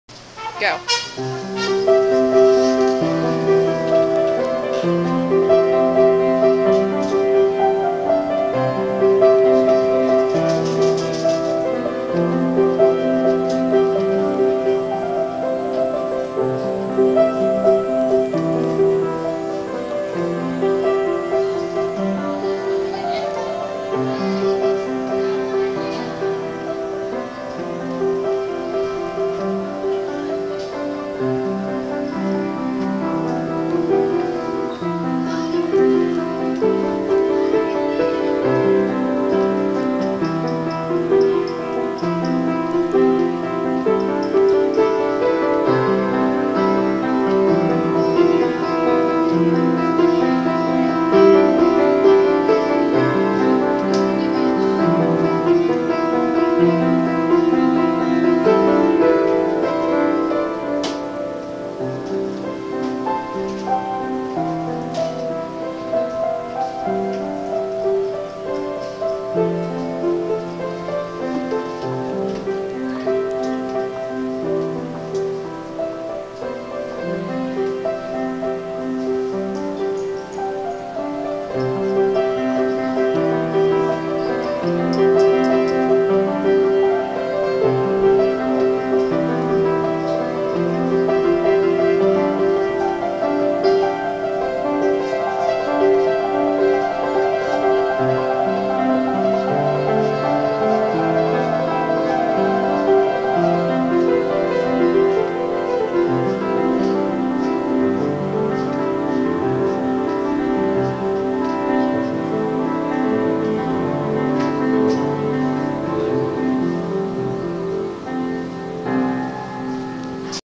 Matthew Caldwell~Original Piano Piece
Matt playing a song he wrote on the piano.
His account is Nlhsbandkid if you want to see some of his other work =D

I'm still not sure why my camera was so dark.

Please ignore all the other noises, we were on a break at band practice.